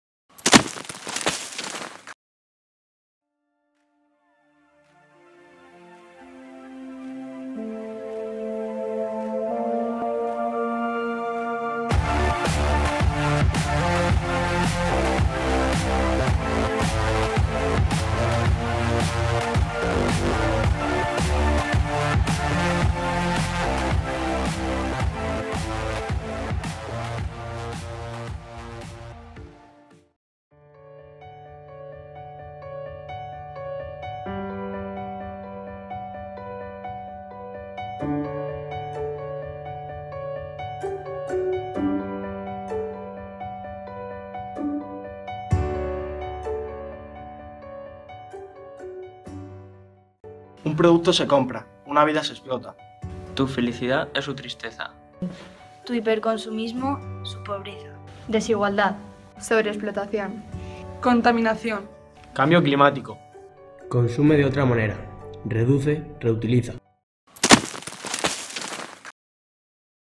DOS VIDAS EN UN HILO
El video comienza con la idea del hiperconsumismo reflejado en una chica adolescente que no se conforma con toda la ropa que tiene. Decide ir de compras y del jersey que se compra sale un hilo que conecta con la realidad de otra vida, la vida de quien realiza esa prenda, y dónde se ve en qué circunstancias se realiza: pobreza, explotación, contaminación, etc.

LA MILAGROSA 4ºB
Festival de Clipmetrajes de Manos Unidas